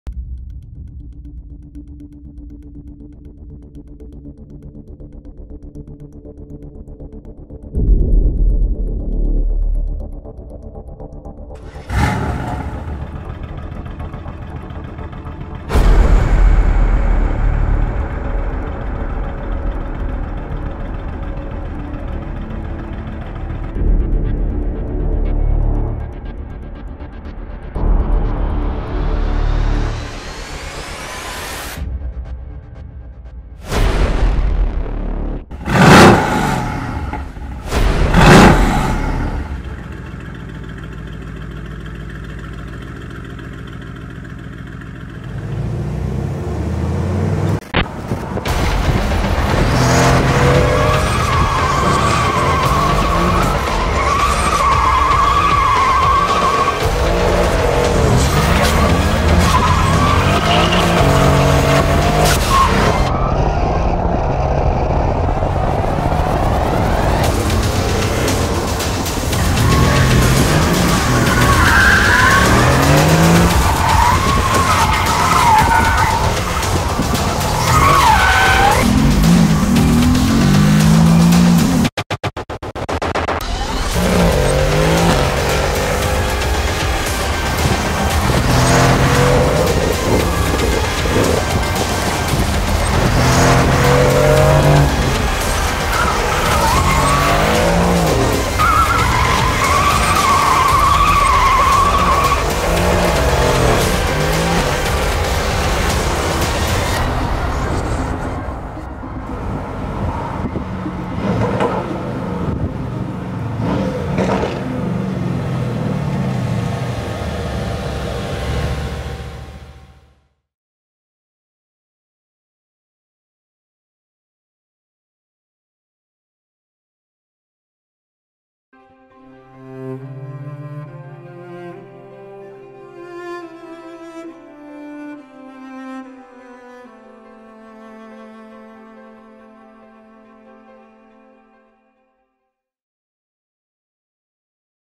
Drifting Mercedes AMG GT-R (Drift from Transformers) 4K
It's a part of a bigger video done for Zachar OFF. Check his YouTube:

Special thanks to professional driver Janusz Dudek for best possible stunts with this car.